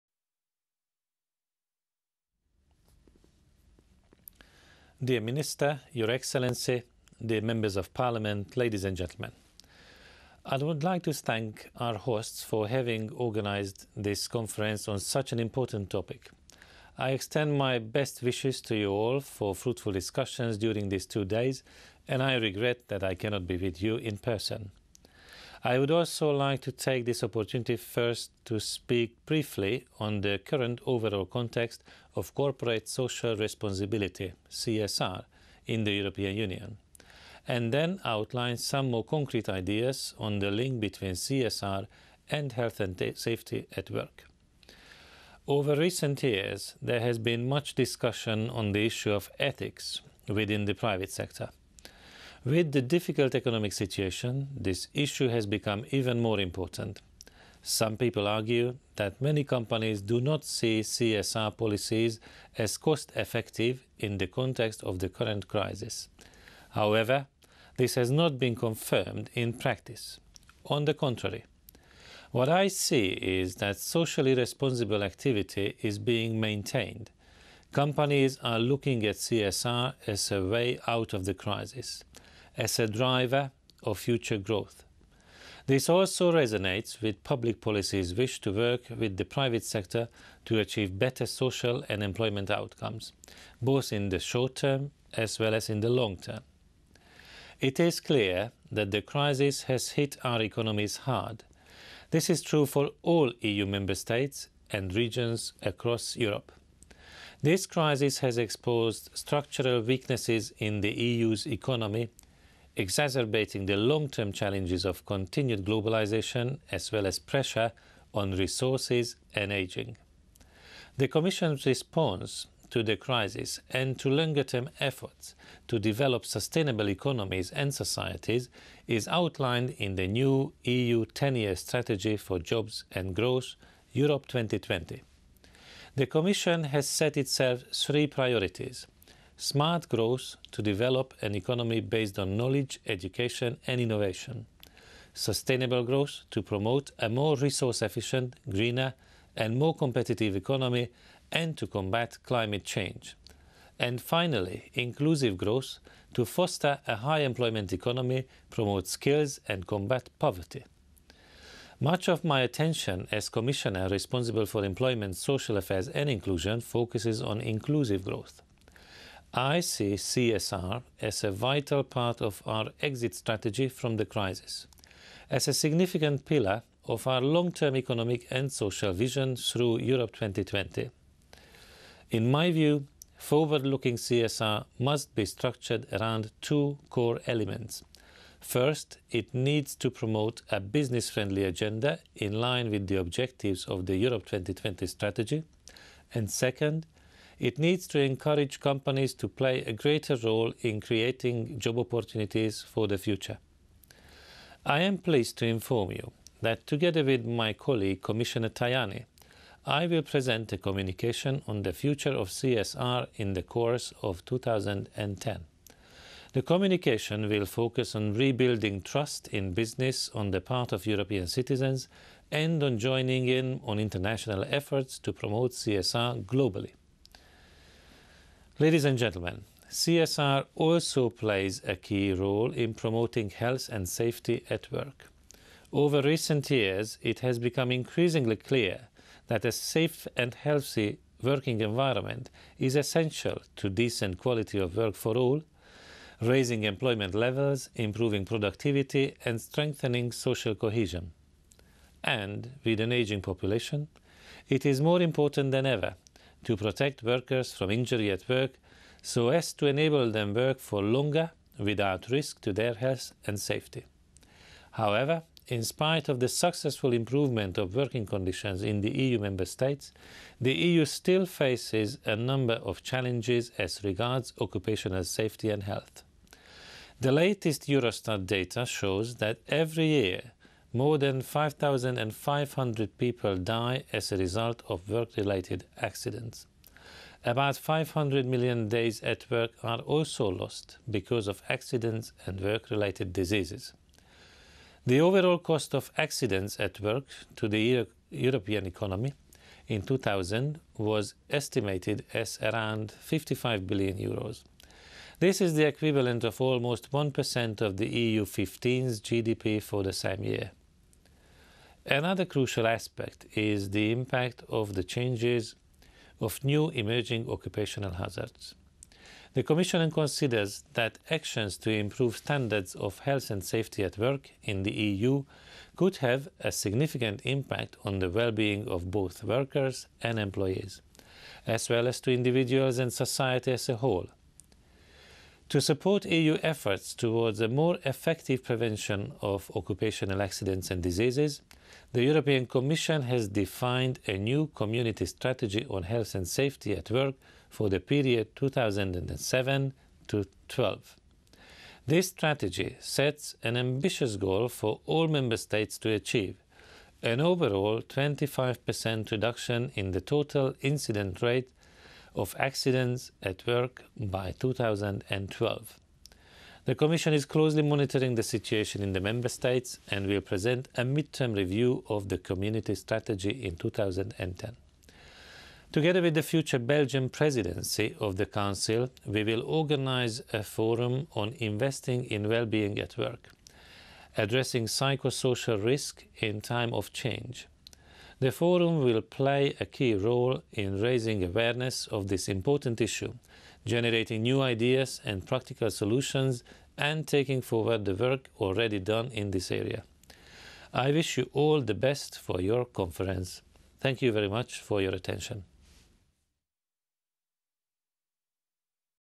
László Andor opening the Fifth Regional Conference on CSR in Sofia, 2010
Speech of László Andor, EU Commissioner for Employment, Social Affairs and Inclusion, European Commission greeting the participants at the Fifth Regional Conference on CSR in Sofia, 2010